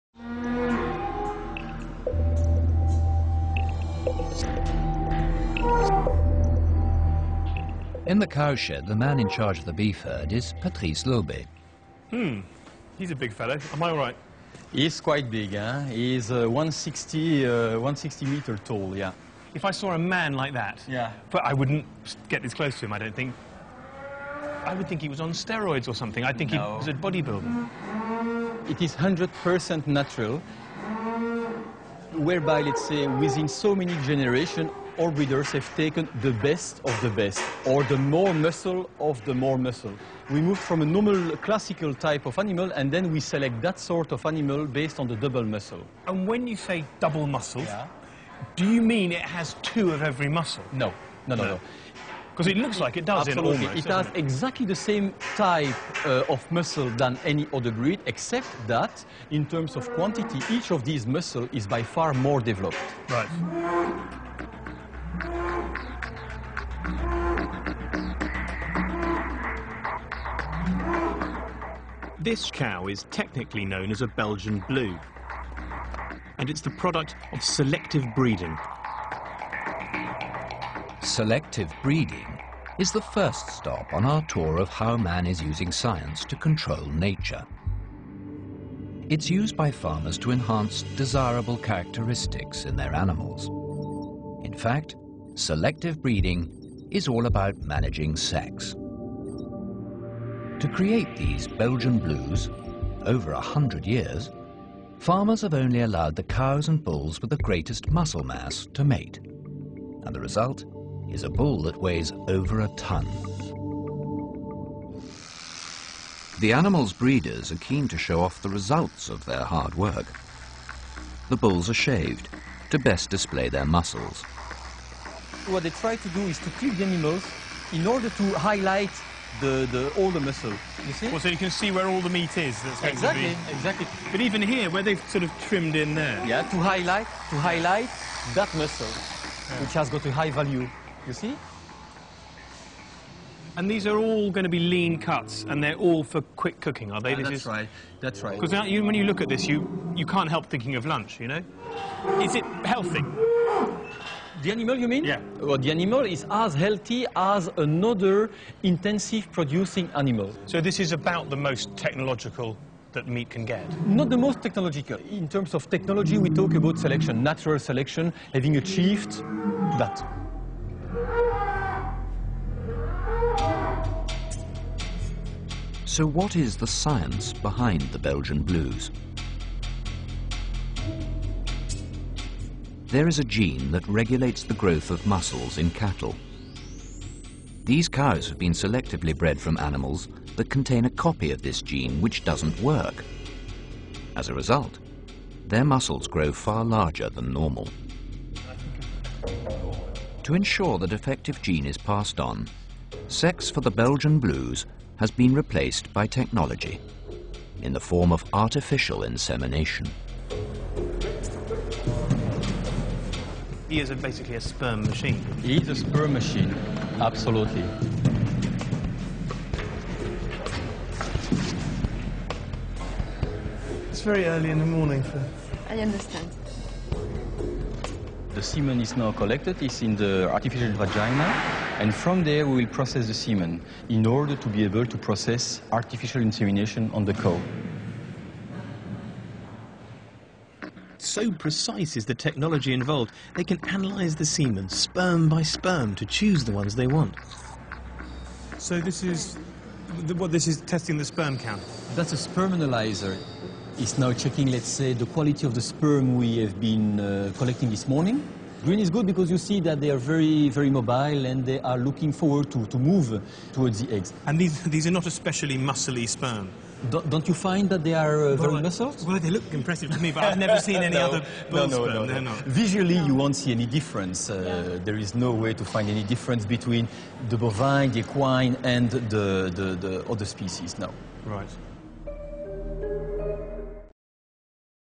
نموذج بقر اللحم المميز | دورة تدريبية في التسمين | حلقة 69
[69] دورة تدريبية في التسمين.ج69.نموذج بقر اللحم المميز (السوبر):
يعرض هذا الفيلم إلى أبقار عملاقة وتوضح هيئة بنائها. ترى كيف حقق المربي نتائج مذهلة ؟؛ كيفية انتخاب وانتقاء العجول المميزة للتسمين. الفيلم يوضح أجزاء جسم الحيوان المختلفة، والعضلات عالية الثمن التي يفضل أن تكون كبيرة الحجم حتى يصبح الإنتاج اقتصادي. حيث تم حلق شعر الحيوان بماكينة القص الكهربائية لإظهار منحنيات وثنيات الجسم المختلفة لإظهار القطعيات المختلفة من لحم الأفخاذ والظهر والبطن والكتف والرقبة. كل ذلك عن طريق التربية بالانتخاب الطبيعي، الذي يؤدي إلى انتخاب أفضل الجينات التي تؤدي إلى أفضل أداء في الإنتاجية وزيادة الأجزاء المميزة في جسم حيوان التسمين.
يظهر كذلك في الفيلم عملية جمع السائل المنوي من أفضل الطلائق المختارة والمنتخبة وذلك عن طريق المهبل الصناعي، لاستخدامه في تلقيح الإناث للحصول على عجول إذا صفات وراثية مميزة، وذلك بتلقيحها صناعيا بعد فحصه (فحص السائل المنوي: الحي والميت، الطبيعي والشاذ، الحركة والحيوية، المذيل وعديم الذيل...وهكذا) وتخفيفه بالمخففات.
ترجمة: محمود سلامة الهايشة (Mahmoud Salama El-Haysha)
Training course in fattening Farm animals.. Part 69